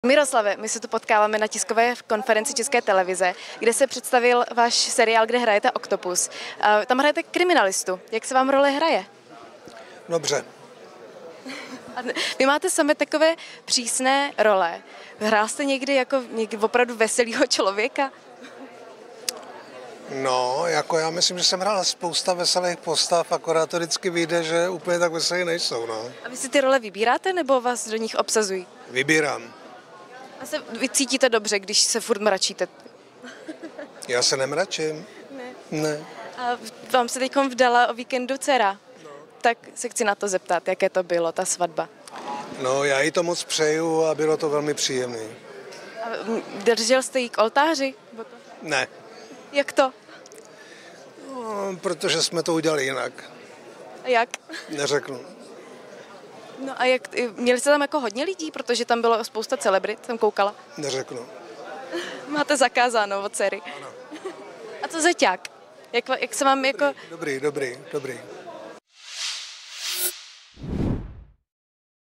Miroslav Krobot promluvil o svatbě dcery: Proč ji nevedl k oltáři? Udělali jsme to jinak, říká
Herec Miroslav Krobot (71) byl o víkendu jedním z hlavních hostů na tajné svatbě své dcery Lenky Krobotové (46). Veselka se prý vydařila, jak Krobot popsal v rozhovoru pro eXtra.cz. Zároveň ale dodal, že ji nevedl k oltáři, jak to u otců nevěst bývá zvykem.